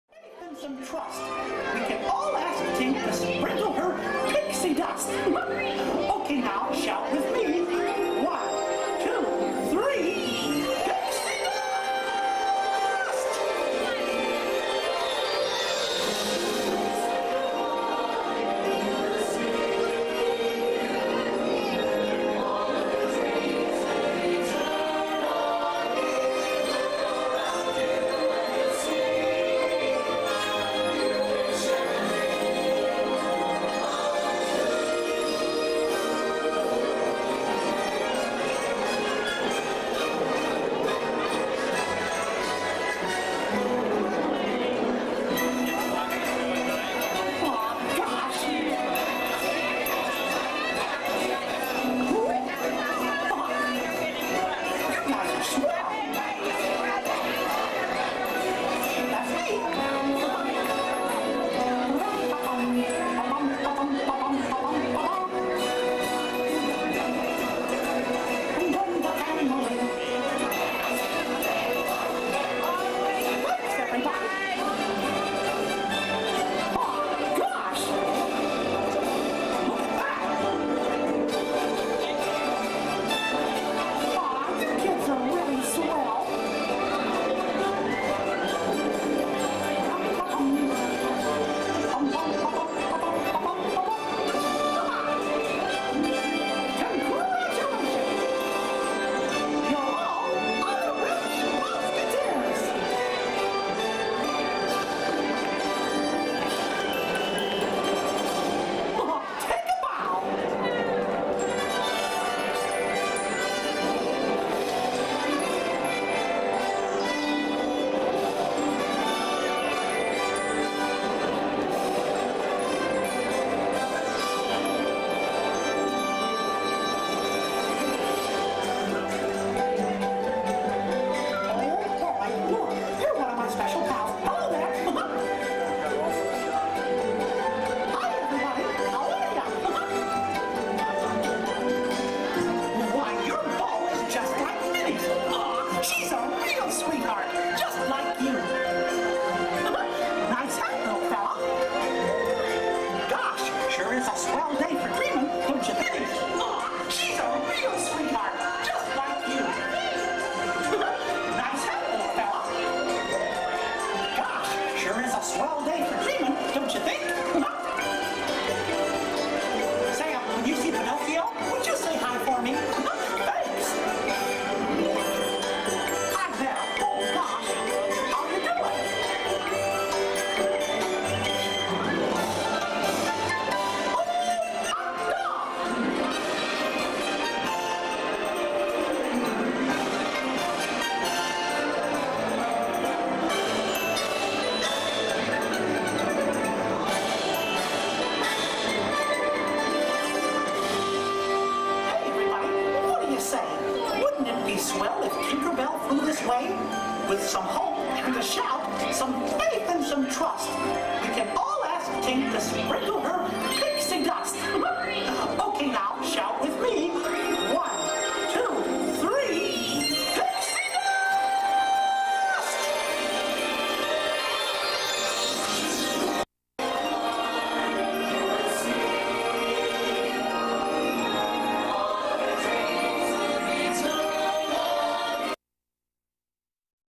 2003年【シェア・オブ・ドリームス】マジックキングダム Walt Disney World Resort MK Parade
フロリダ・ウォルトディズニーワールドリゾートのマジックキングダムで開催されていたパレード「シェア・オブ・ドリームス」の様子です。